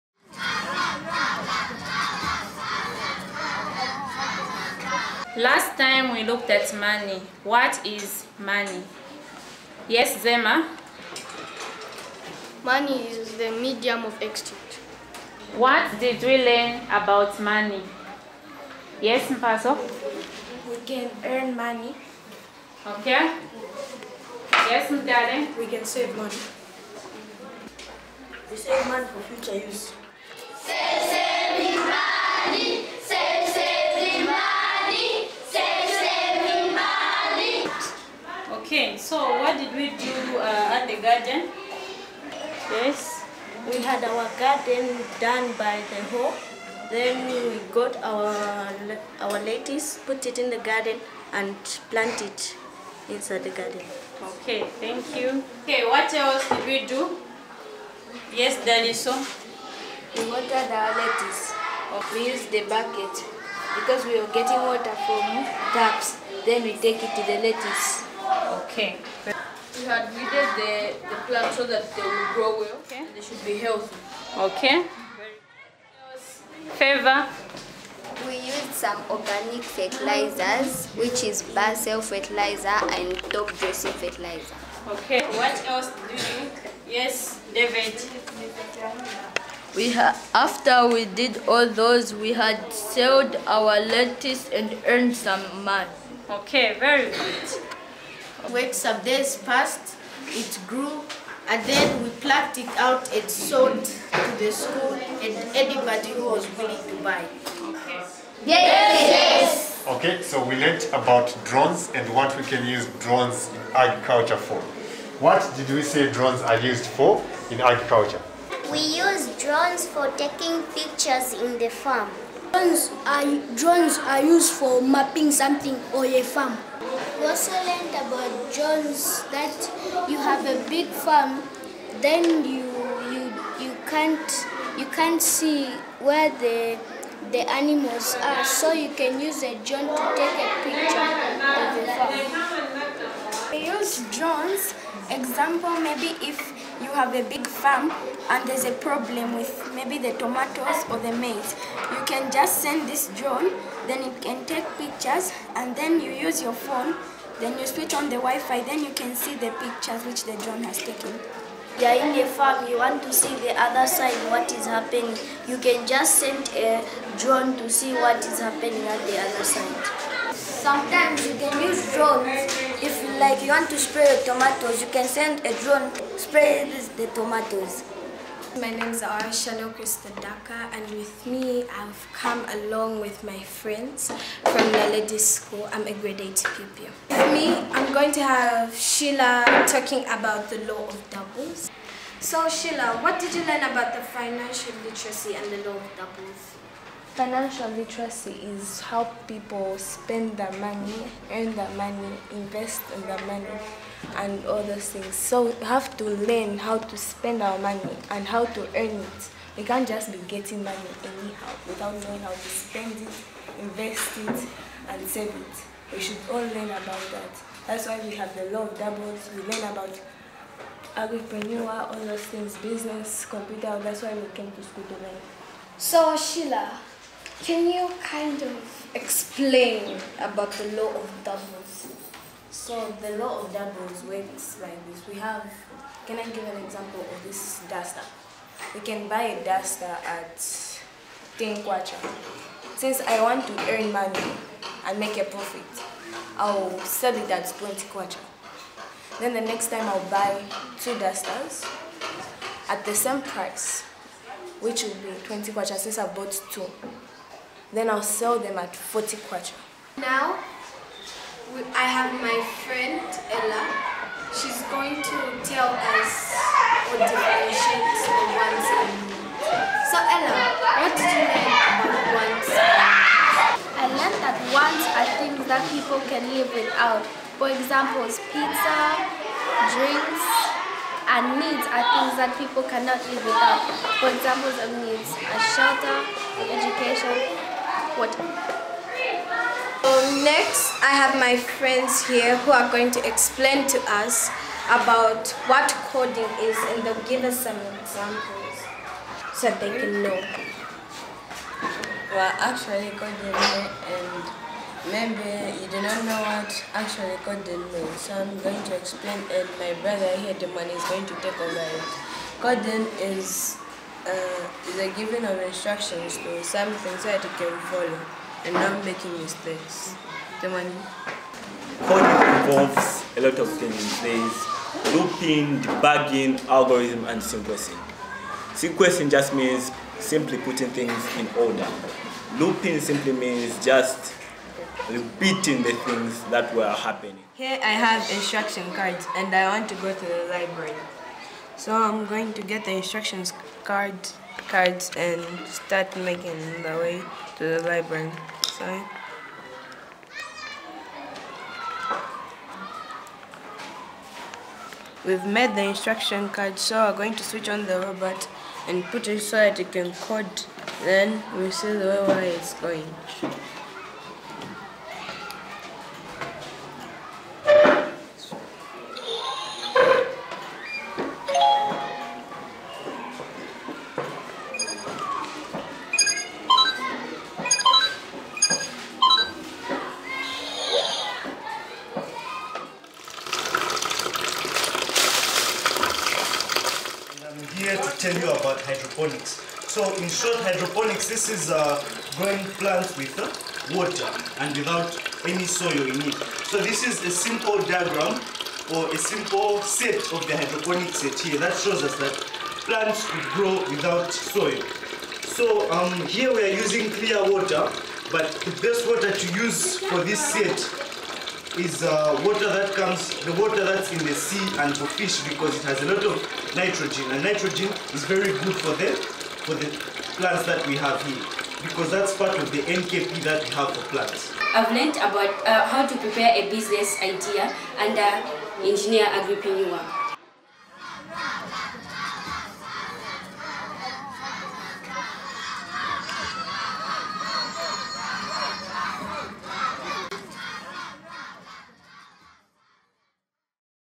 Junior Agripreneur Documentary: Naledi School
Agrireneurs Summarise their take away lessons. These Skills will help them create a better community.